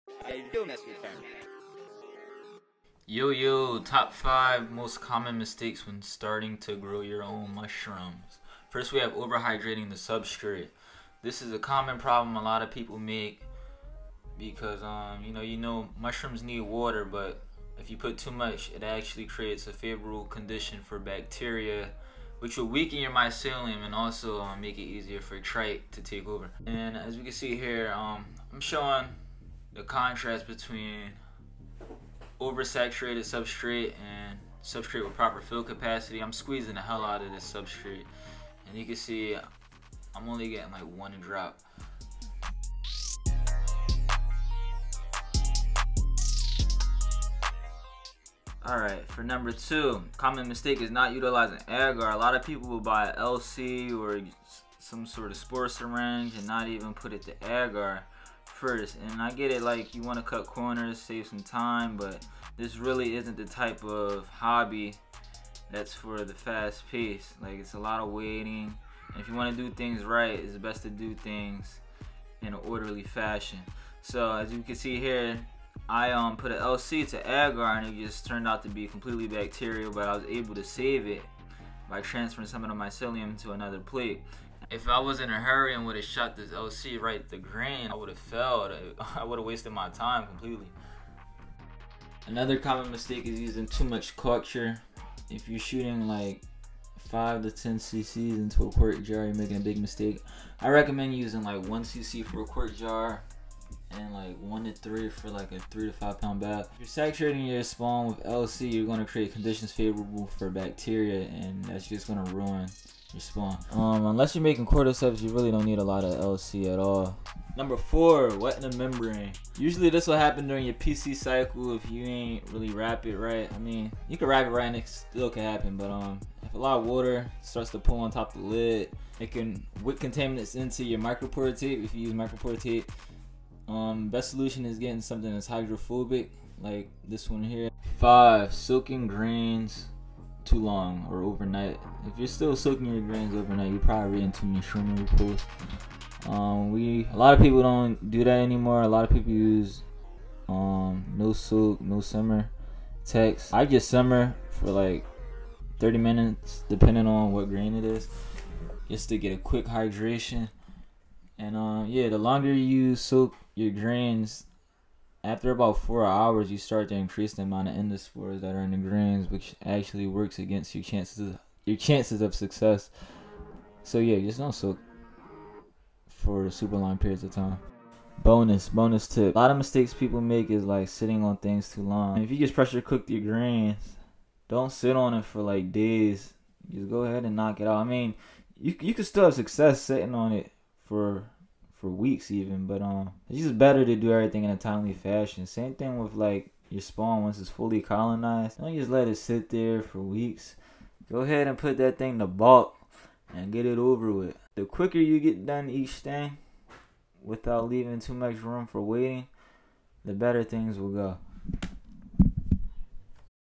Top 5 Mistakes Growing Mushrooms
How not to grow mushrooms.
My top 5 common mistakes when growing mushrooms.

If there are any that you think should have been mentioned please put it in the Comments below ⤵️.

1. Over Hydrating Substrate
(literally squeeze tests should produce 1-2 drops, over multiple attempts.)

2. Not utilizing Agar
(Don't just put LC straight to grain, test it out, I've brought LC from many reputable people in this community only to be let down by bacterial cultures.)

3. Using to much Culture
(1 cc for a Qt Jar of spawn, 1-3 for 3-5lb Bag of spawn, Less is more)
(Also for agar don't skimp on the agar when inoculating grain use a good amount (at-least 3 quarter sized pieces) for a quart size of grain)

4. Wetting Membrane on Jar's Lid
(Solution: flip lid upside down or get a hydrophobic gas exchange patch)

5. Soaking Grains for to long
(Soaking overnight may increase amount of endospores in grain reducing your chances of success).